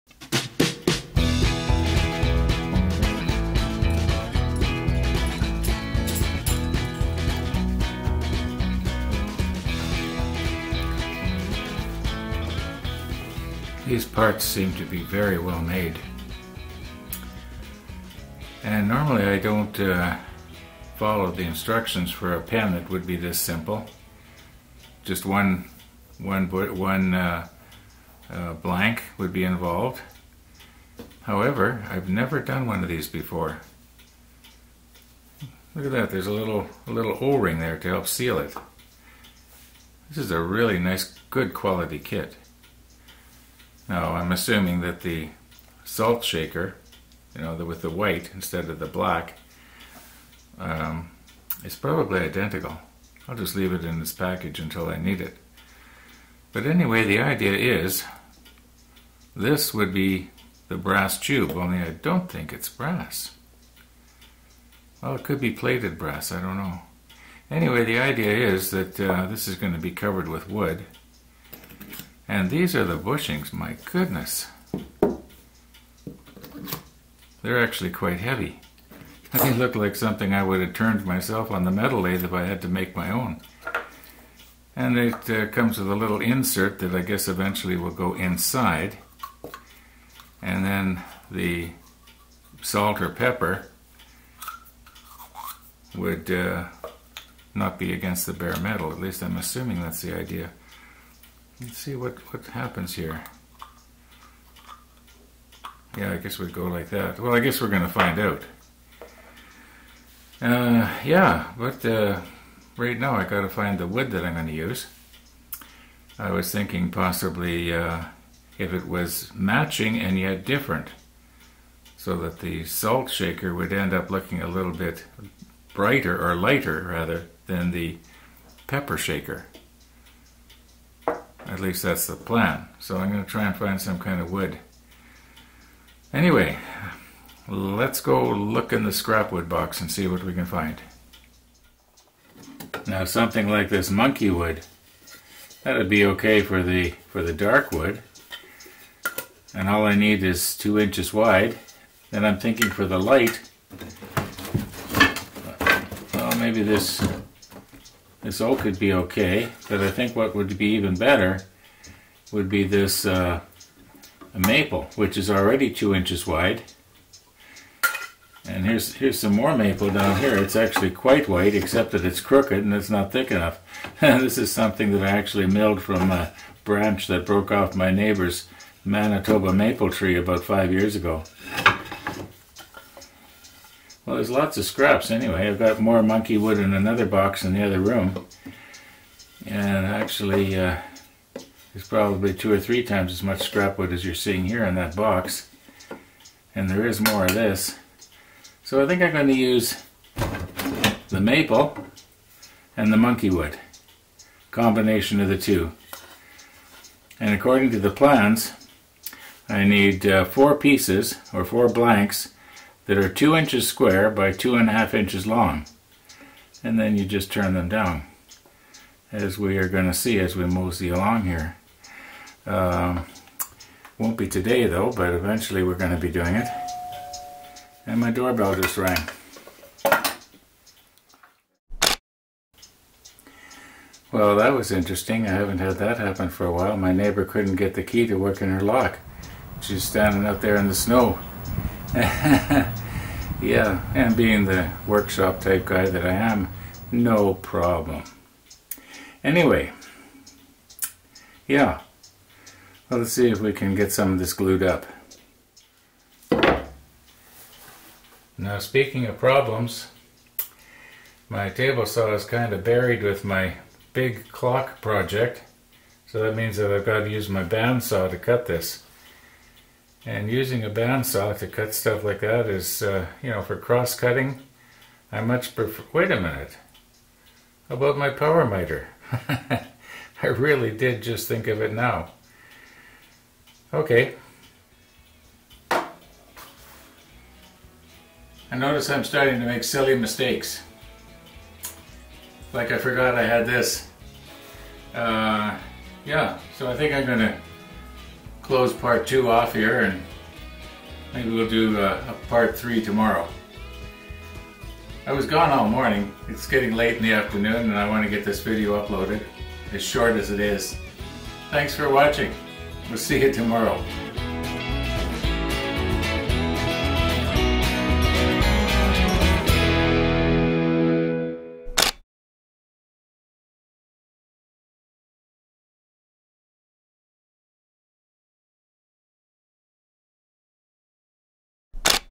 The 10th Free Draw - Part 2
This “Free Draw” is open only to members of the “Manitoba CAN-RC and URC, buy & sell” FaceBook Group living in the Winnipeg and Carman area, and is limited to the first 60 who respond.